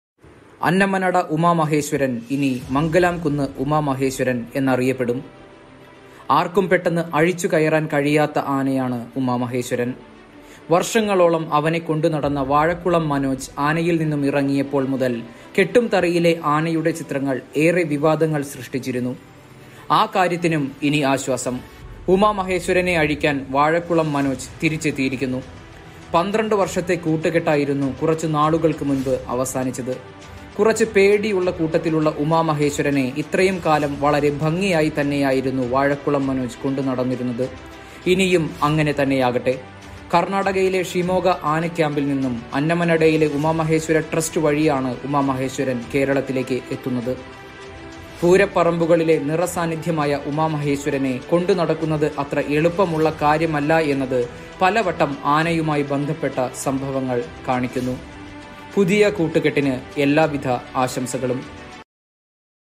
വാഴക്കുളം മനോജ് തിരിച്ചെത്തി. പുതിയ പേരിൽ മംഗലാംകുന്ന് ഉമാമഹേശ്വരനും | uma maheswaran and Manoj
Annamanada Uma Maheswaran now renamed to Mangalamkunnu Uma Maheswaran and his earlier mahout vazhakkulam Manoj taken charge for him again which is news that gives relief.their elephant mahout relationship was having a length of 12 years and he has been moved out of his responsibility few months back .now he is taken charge again.